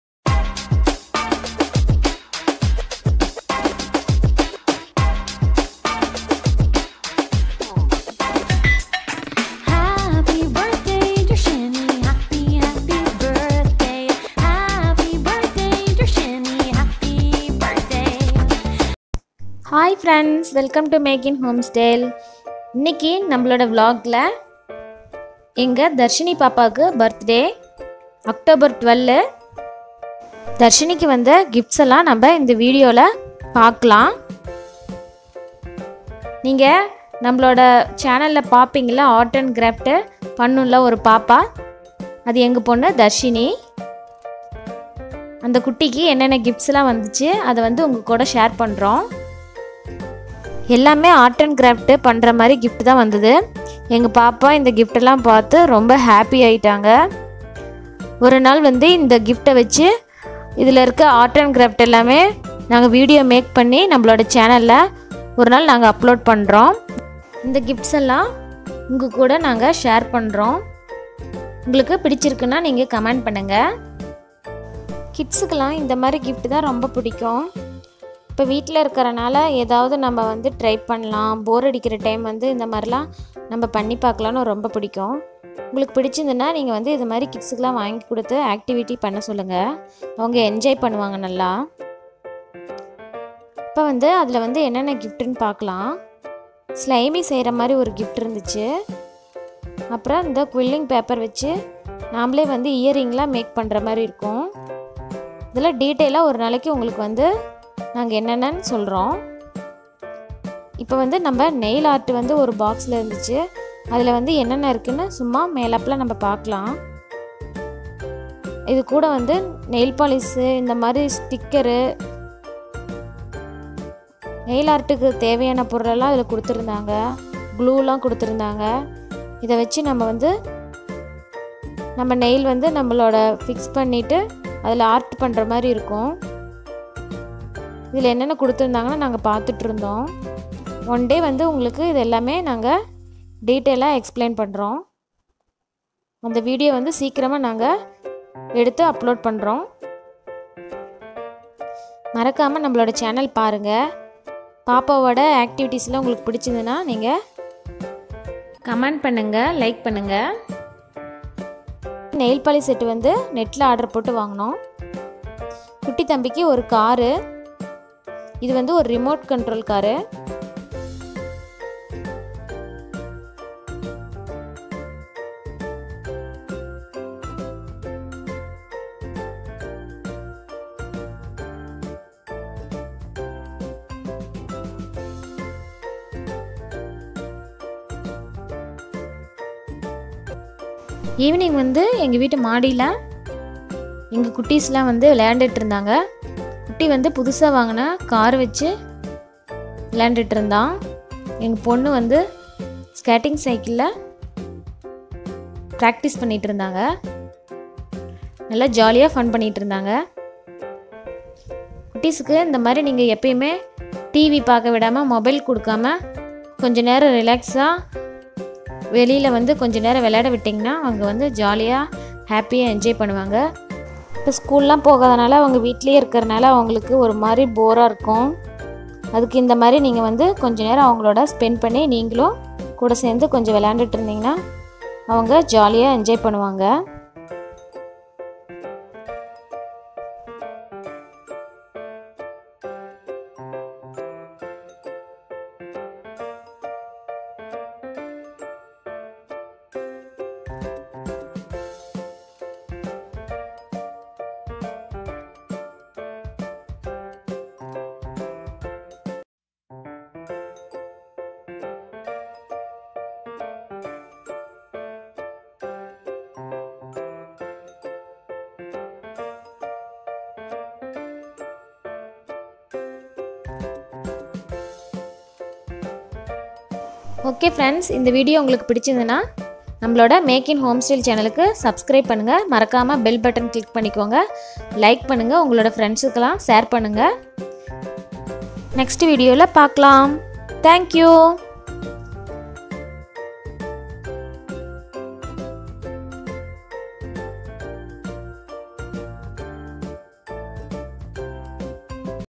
Birthday Surprise for my Daughter | surprise birthday gifts | art and craft fun activities in home
In this video kids like surprise gift and fun game activities in home